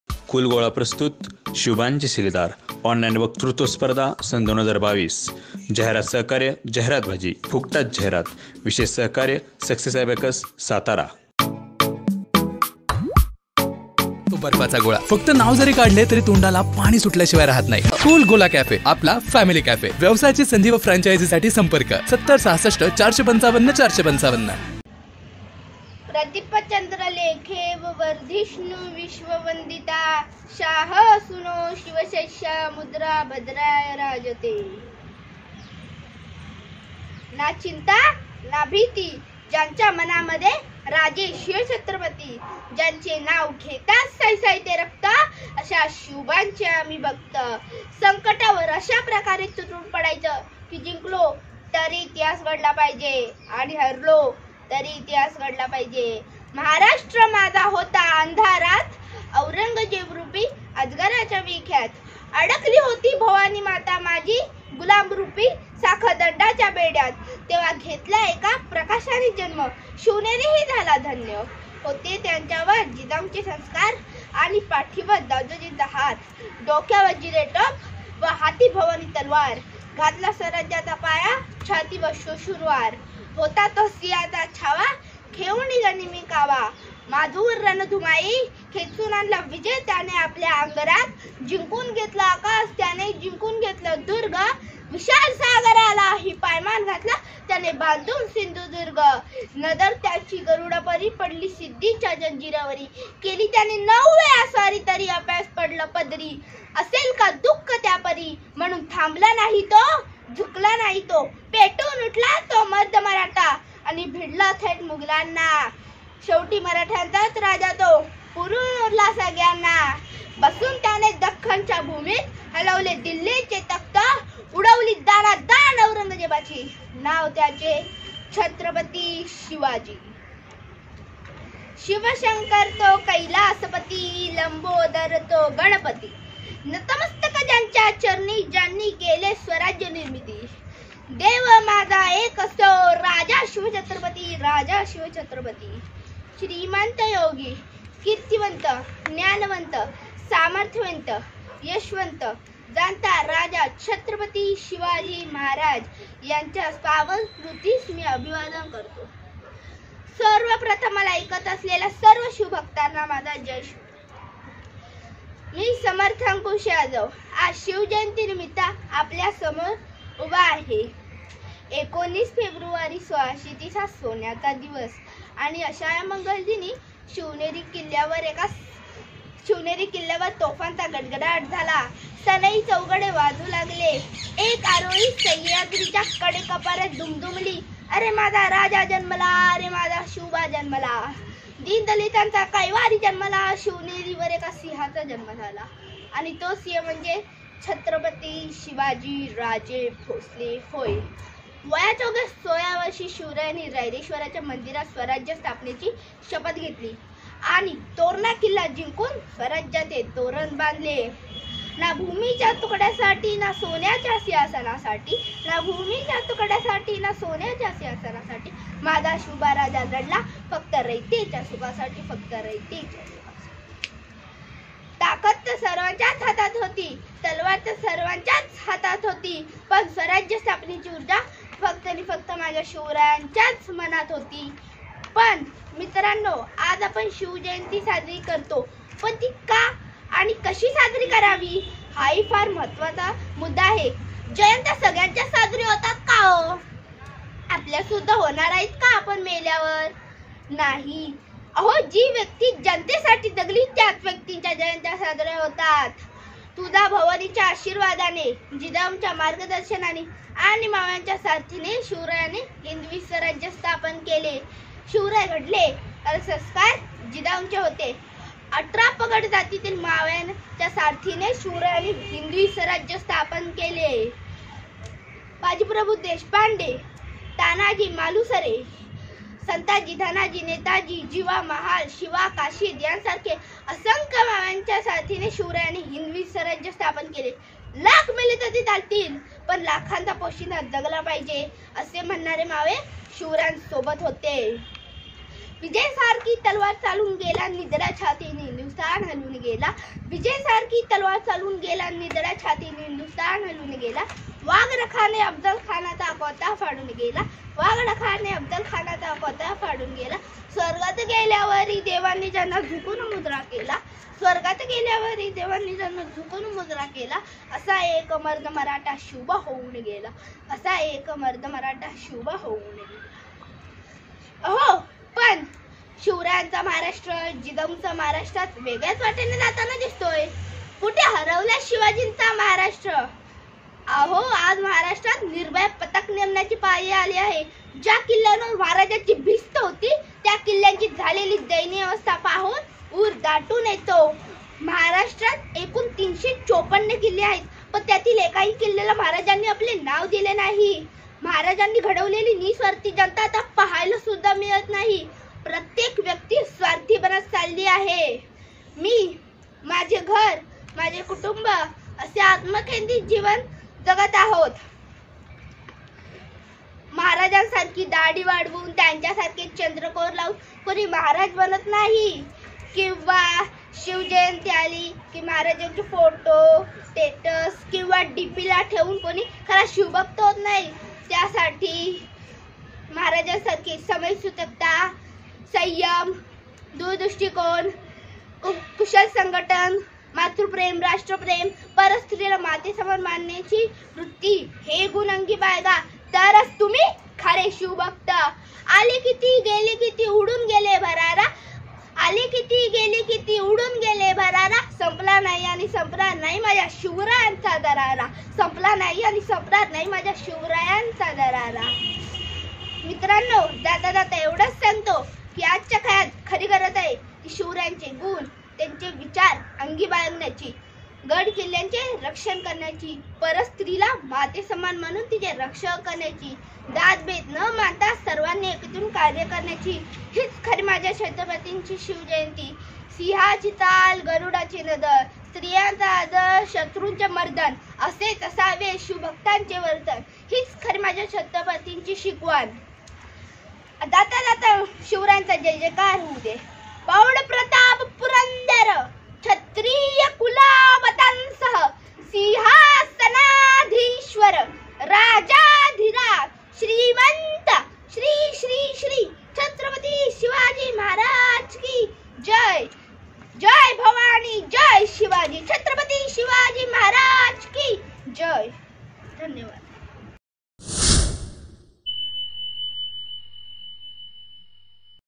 कु. समर्थ यादव | Group-A | शिवबांचे शिलेदार-ऑनलाईन वकृत्व स्पर्धा| CoolGola Cafe | Cool Gola
कुलगोला कॅफे तर्फे घेण्यात आलेल्या "शिवबांचे शिलेदार" या ऑनलाइन वक्तृत्व स्पर्धेत या स्पर्धकाने आपले "शिवविचार" जनसामान्यांपर्यंत पोहोचवण्यास सहकार्य केले..!